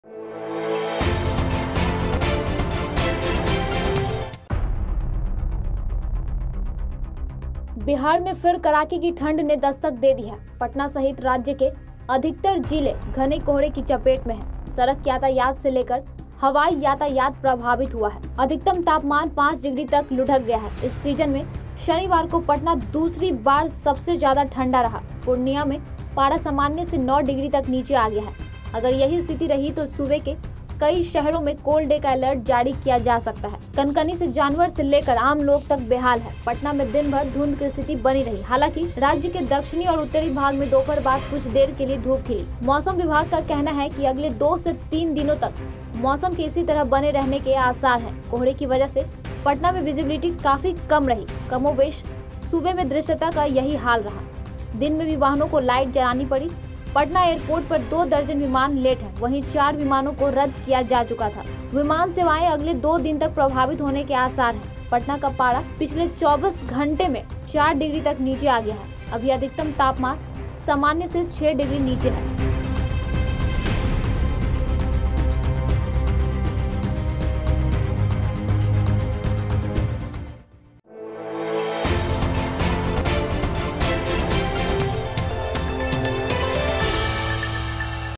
ठंड से कांप उठा बिहार, यातायात प्रभावित
बिहार में फिर कड़ाके की ठंड ने दस्तक दे दी है। पटना सहित राज्य के अधिकतर जिले घने कोहरे की चपेट में हैं। सड़क यातायात से लेकर हवाई यातायात प्रभावित हुआ है।